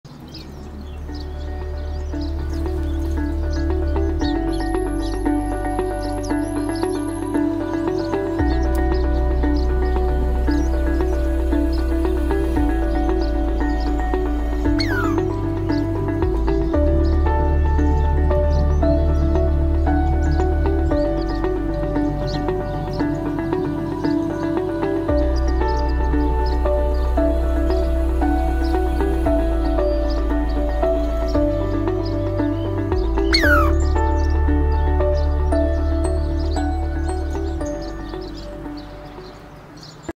Squrriel attacks a bird feeder
Insolent squirrel occupied the bird feeder and ate all the food!